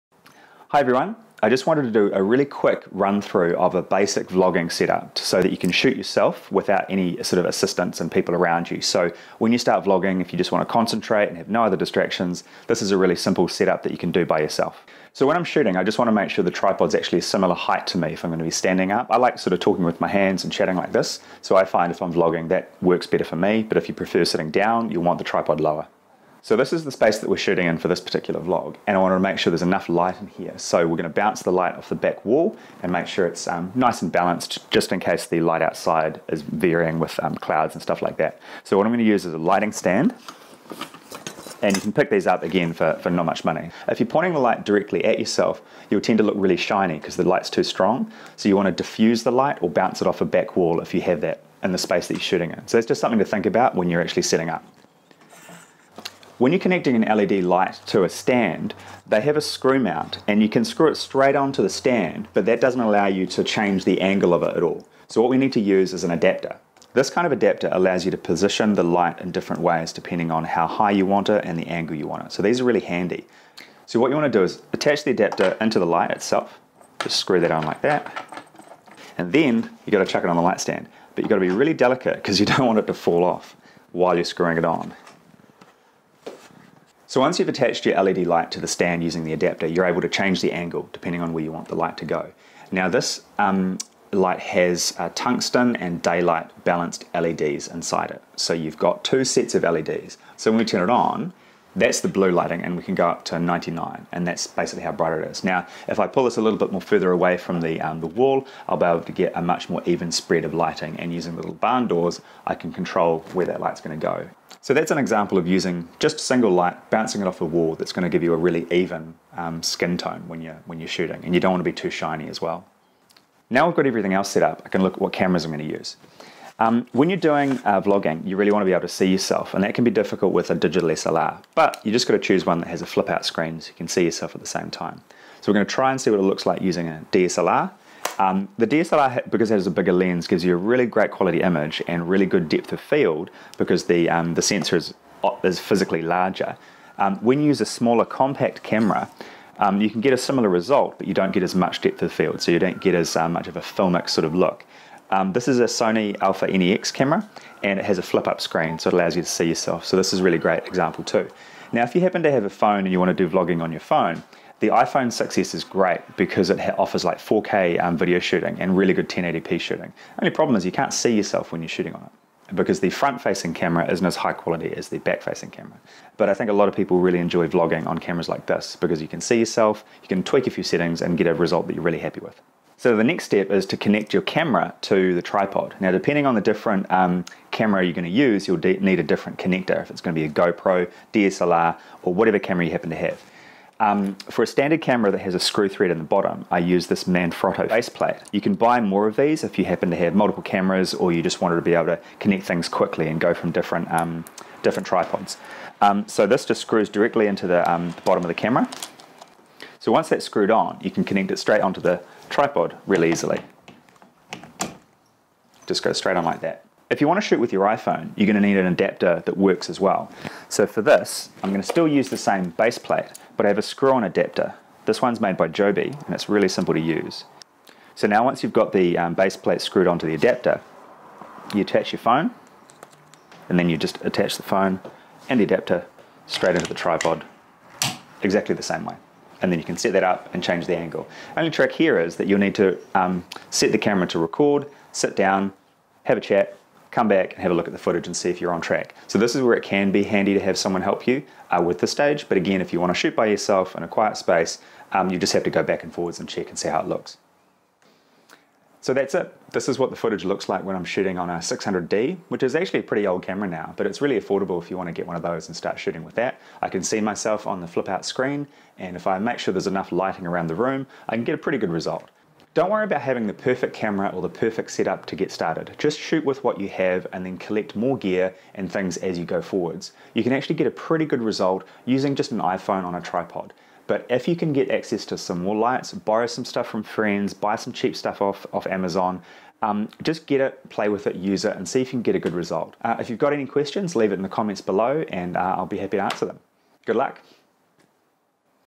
Tutorial: Basic Vlogging Setup (with LED lighting)
A quick walkthrough demo of a vlogging setup including a single LED light bouncing off the wall in the lounge.

For this example I've used a Gioto Tripod and Manfrotto head, but you can use any tripod you like as long as you can get the height you need.

Camera examples for this demo:

Canon 600D
Sony Alpha NEX
iPhone 6s

Audio recorded with a Movo lapel mic (check for these on Amazon) running into my iPhone 5s in my pocket. I synced the audio later. It's an affordable mic, but it's actually quite sensitive, and I'd clip this onto someone's shirt, further away than I did in this demo.

You can use your camera or smartphone to record audio, but you will pick up some echo from the room. You will get much better audio from a microphone. You can plug it into the audio input on your camera or smartphone.

Don't worry about trying to get the perfect setup or combination of gear. New gear is always coming out, and some of your old gear will decide to randomly stop working - just work with what you have, keep practicing and making videos.

(I included the on-screen title twice during the video to try and hit the automatically generated thumbnails that YouTube create. You will need more than 100 subscribers before you can create or set your own thumbnails.)